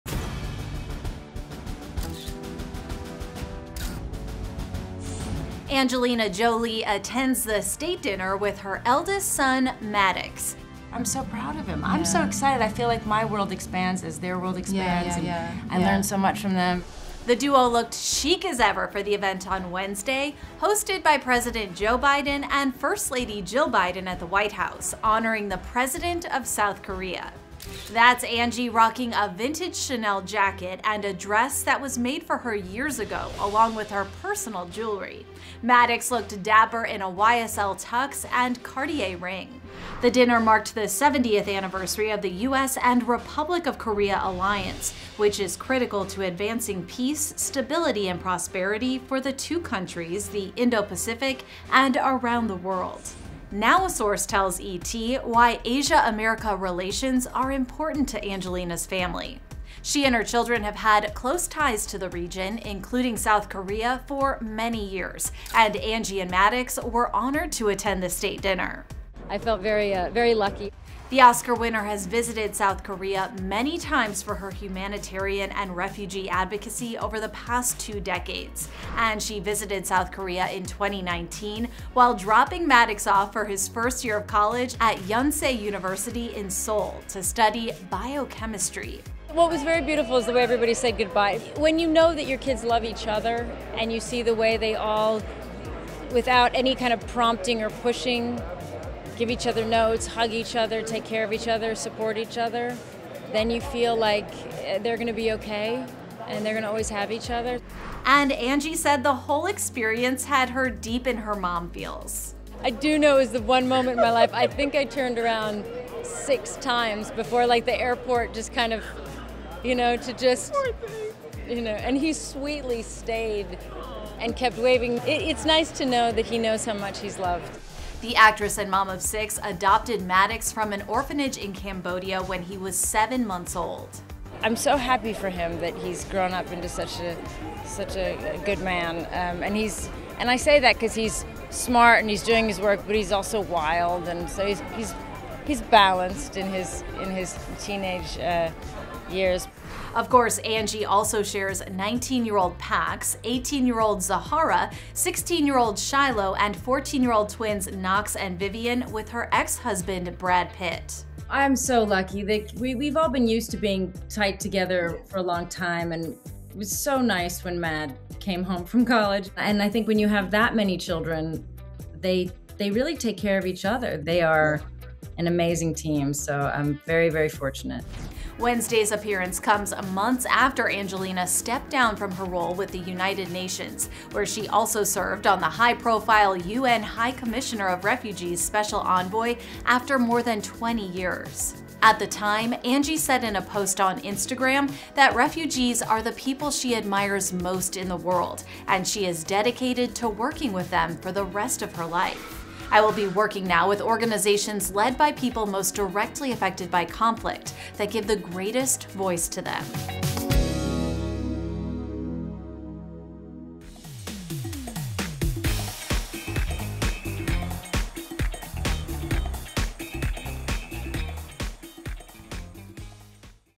Angelina Jolie and Son Maddox Make RARE Public Appearance
Angelina Jolie attended the State Dinner hosted by President Joe Biden and First Lady Jill Biden at the White House with her son, Maddox Jolie-Pitt, on Wednesday.